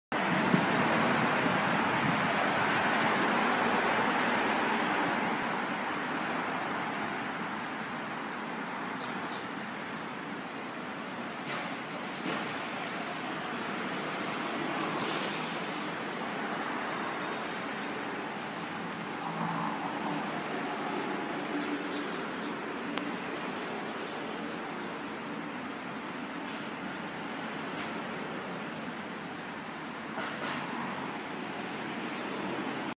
Walking in Zhengzhou
Robert looking around in China November 6, 2017.